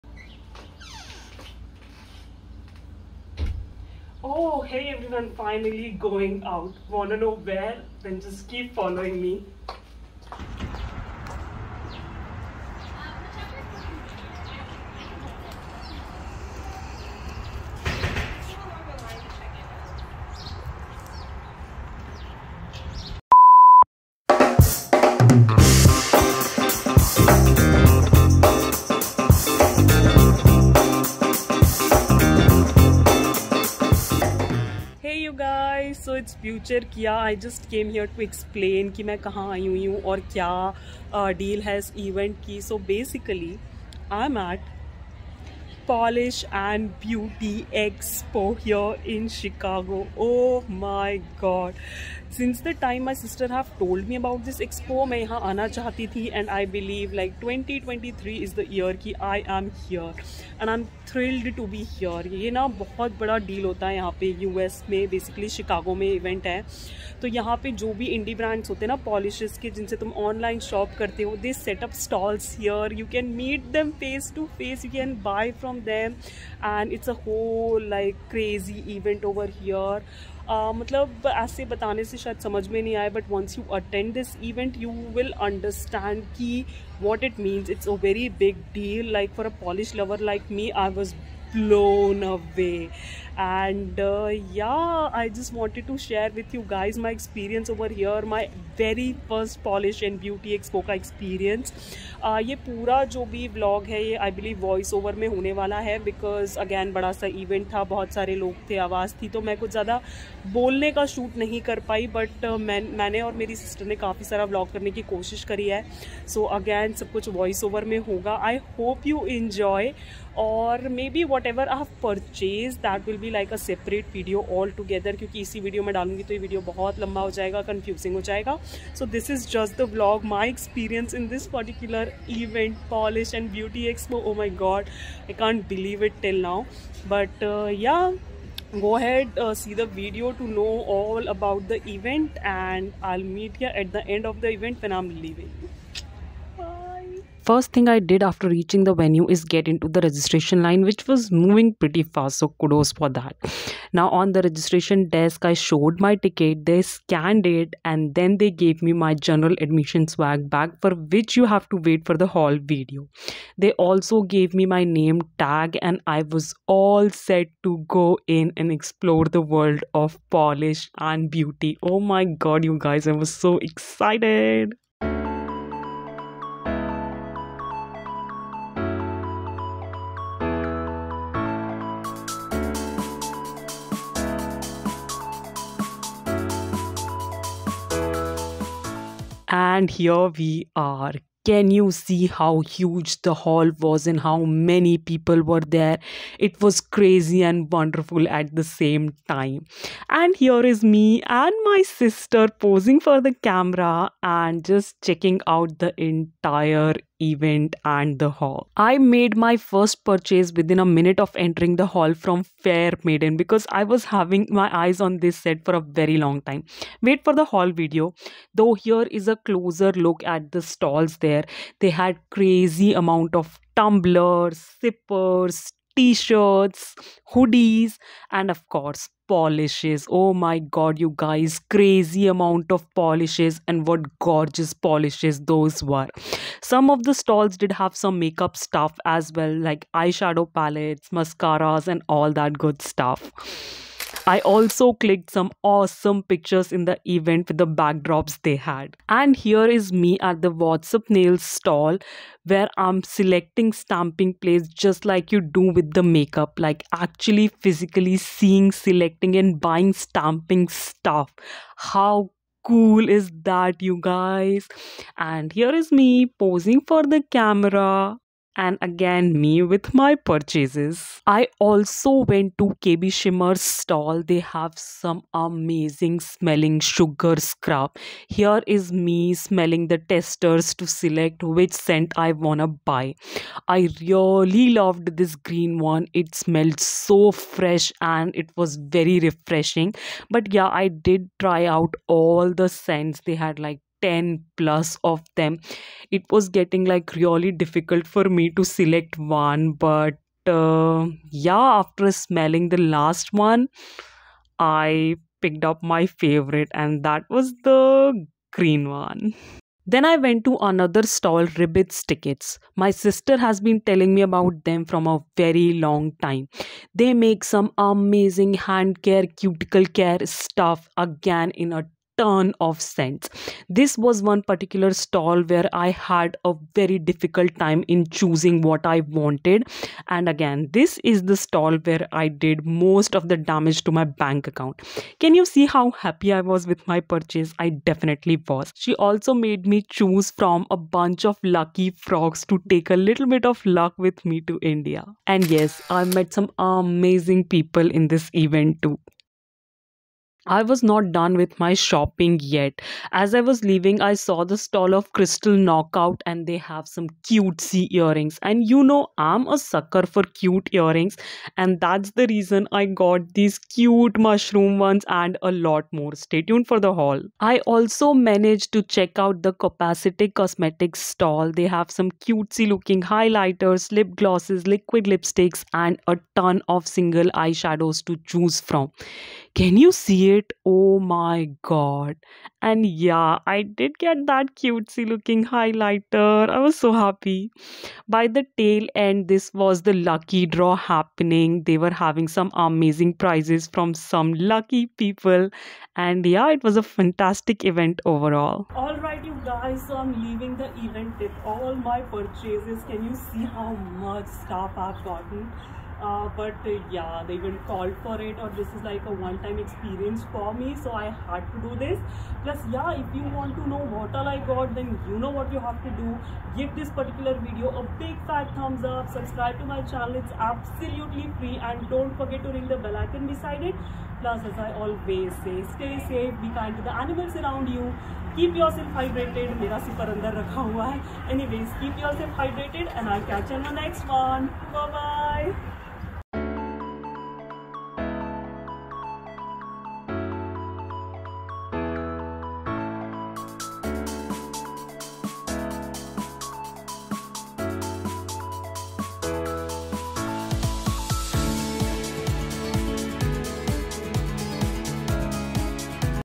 Polish & Beauty Expo 2023 Vlog: An Unforgettable Experience
Hey Everyone,
I had been hearing about Polish & Beauty Expo from my sista for the past couple of years n finally I got a chance to fly down to the States all the way from India to attend one in Tinley Park, Chicago!
Come along in this vlog for a crazy and wonderful ride and share the experience with me! :D